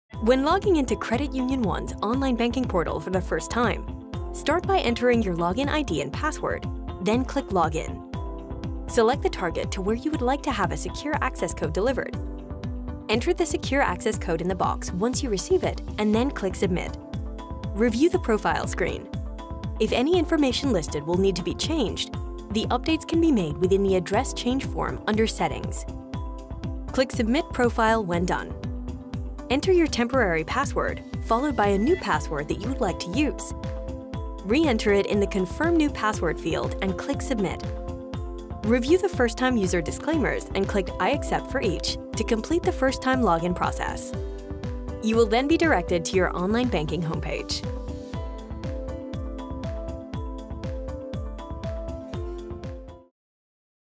How to Log In to Credit Union 1 Online Banking
Follow these steps to log in for the first time to Credit Union 1’s online banking portal and confirm your account information is correct.